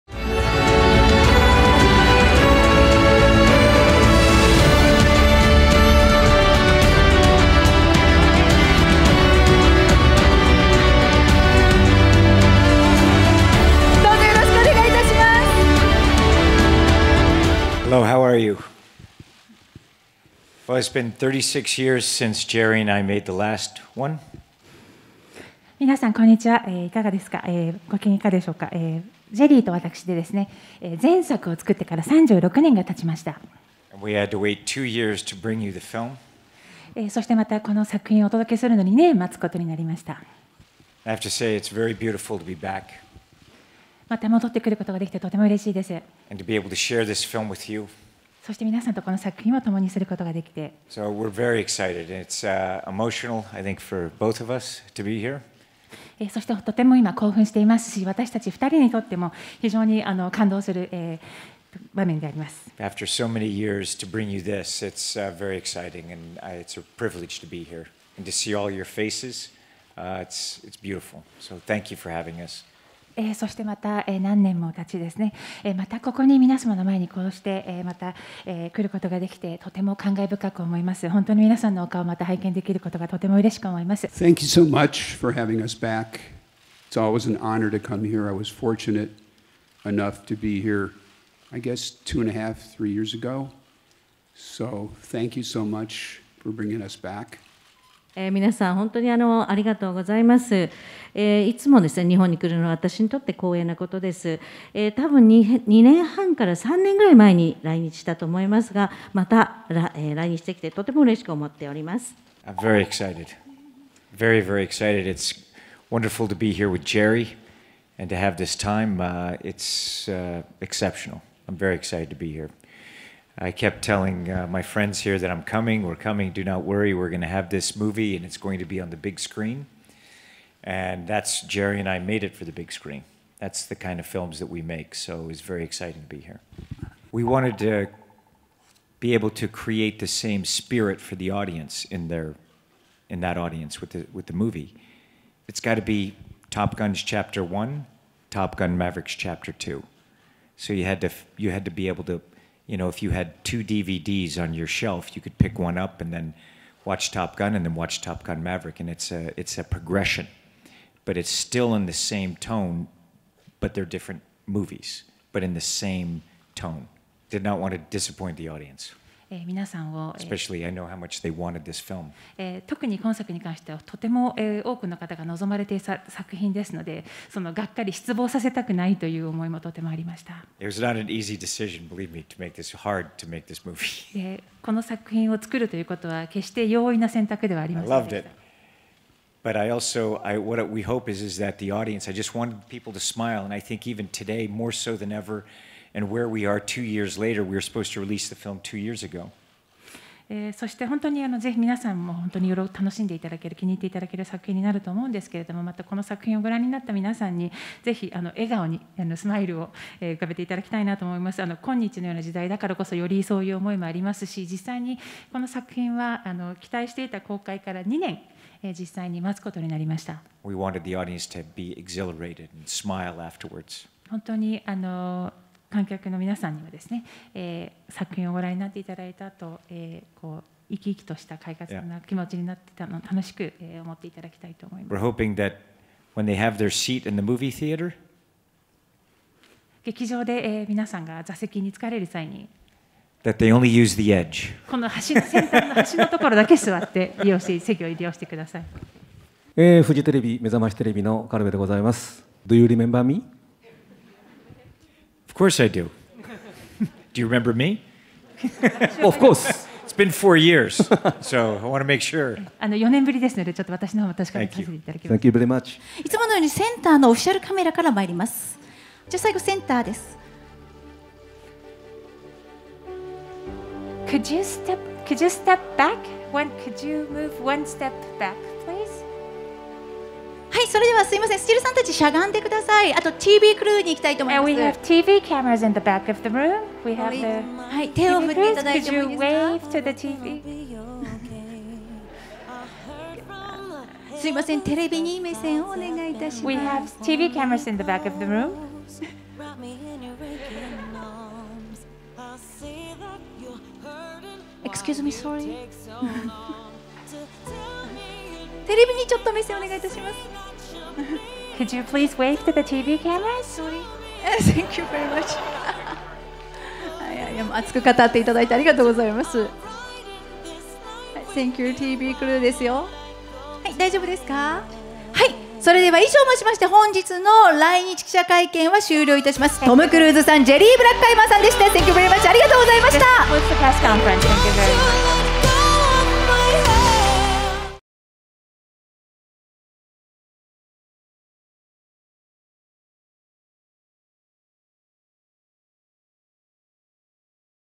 トム・クルーズ、3年10か月ぶりの来日に満面の笑顔／映画『トップガン マーヴェリック』来日記者会見
映画『トップガン マーヴェリック』トム・クルーズ来日記者会見

『トップガン マーヴェリック』公開を5月27日に控え、主演のトム・クルーズが来日記者会見を行った。
トム自身も特に思い入れの深いタイトルである『トップガン』、その36年ぶりの新作を携えての来日とあって、「前作から36年が経ち、この作品をお届けするのに(コロナ禍で)2年待つことになりました。

何年も日本に来られていませんでしたが、また必ず日本に行くからね！とずっと言ってきたので、この作品と共にまたこうして来ることができて感動していますし、皆さんに観ていただけるのでとても興奮しています。」と感無量の様子でコメント。
「まるで戦闘機の中にいるような新しい体験をしていただけるのはもちろん、
私たち2人は、ストーリーへの愛、映画への愛を込めて、”生きること”を讃えるような映画になりましたし、必ず皆さんを楽しませる作品です！」と”胸熱”ポイントをトム自身の口から力強く語った。

予定よりもたっぷりと話しつくしたトムとジェリーは、会見の最後に写真撮影のために立ち上がったものの、カメラに手を振ることを忘れステージ上でしばらく2人きりで話し込んでしまい、呼びかけに気づいたトムが「あ・・・！ソーリー、失礼しました(笑)」とおどけて会場を沸かせる一幕もあったりと大盛り上がり！
いよいよ本作の公開に向けたクライマックスの到来をまじまじと感じさせるビッグイベントとなった。

■来日直前のトム・クルーズ「待ちきれない」と胸熱コメント／映画『トップガン マーヴェリック』